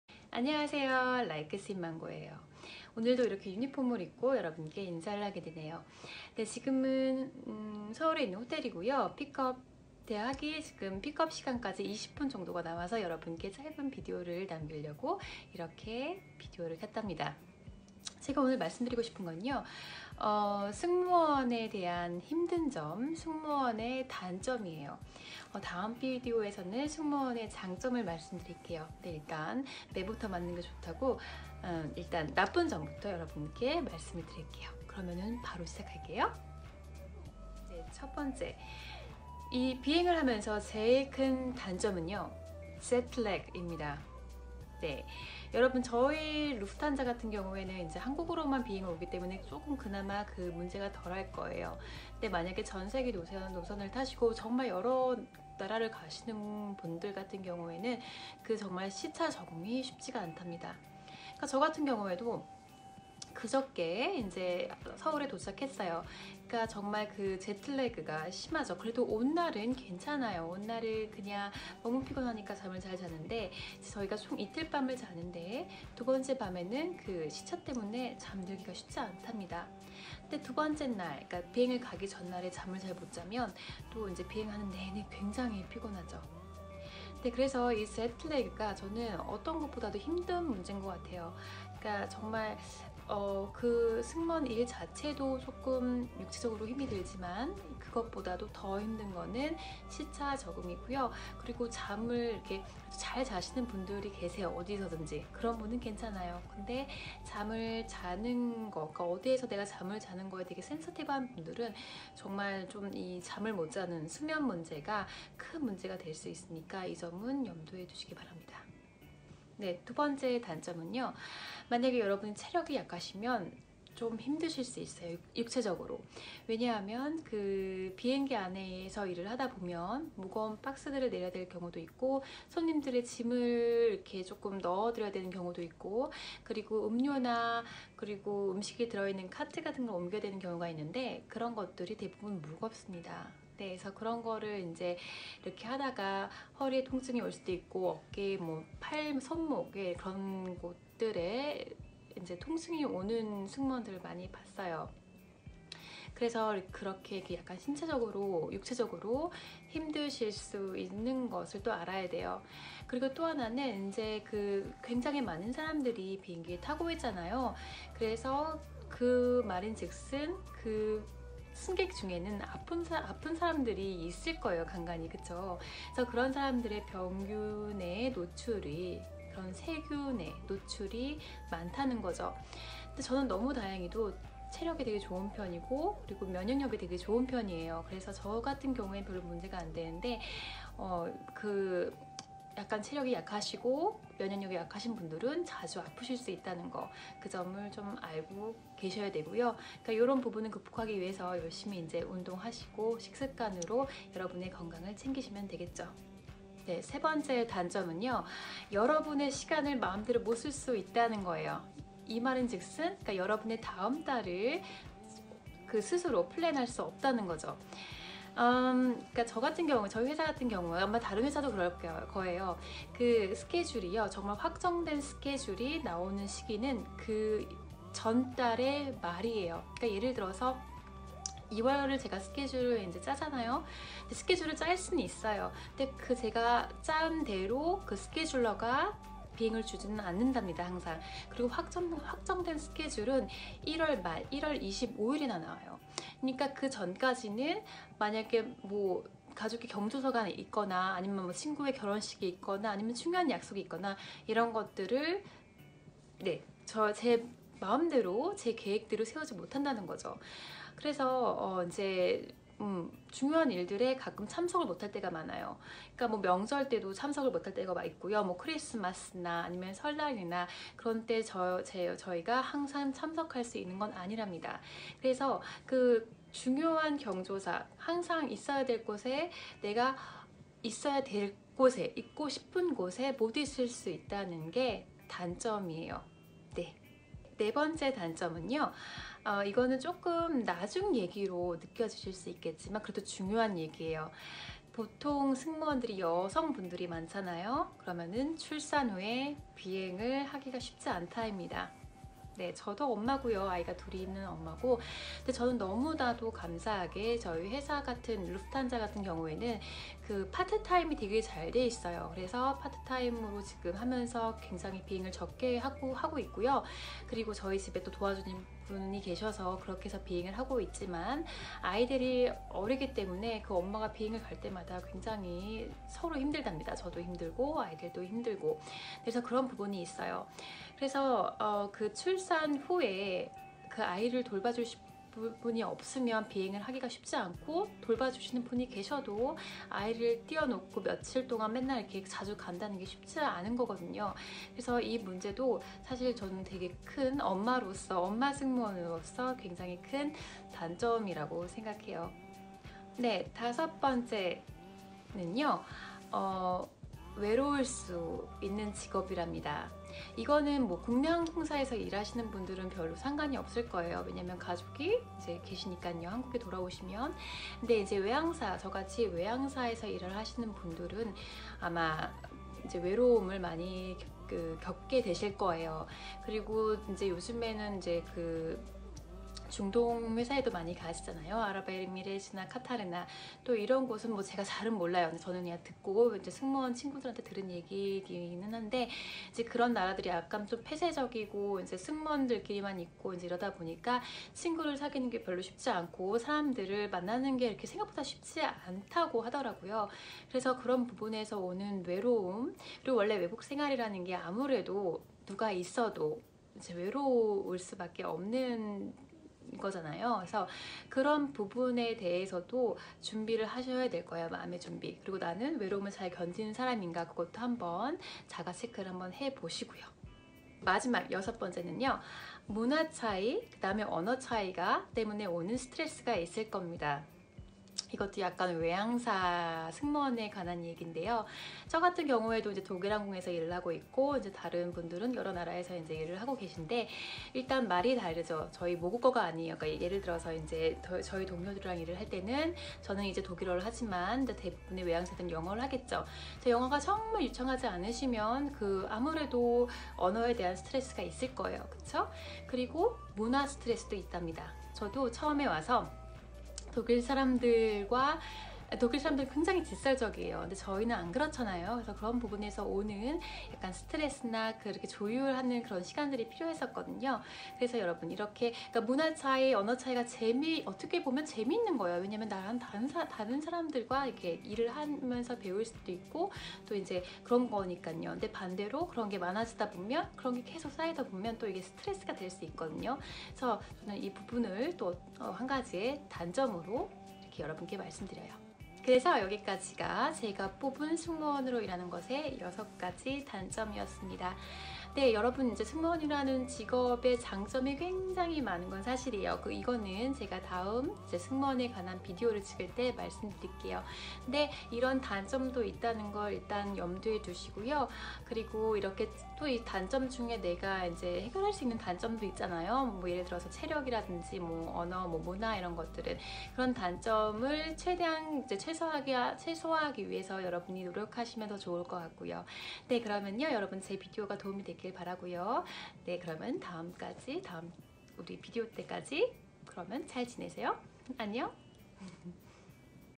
[외항사 승무원] 알려주는 승무원으로 일 하는 것의 힘든 점(단점)
안녕하세요? 오늘은 승무원으로 일 하는 것의 힘든 점을 알려드려요. 승무원이라는 직업이 겉으로 보기에는 화려해 보이기 쉬운 직업이지만, 사실 저희 나름의 힘든 점이 많답니다. 그래서 이 직업을 오래 하시는 분들도 많지 않은 거 같고요. 승무원이 되기를 꿈꾸는 분들에게 저희 직업의 단점을 알려 드려요. 이 단점을 보완하기 위해서 미리미리 준비하면 더욱 좋겠죠?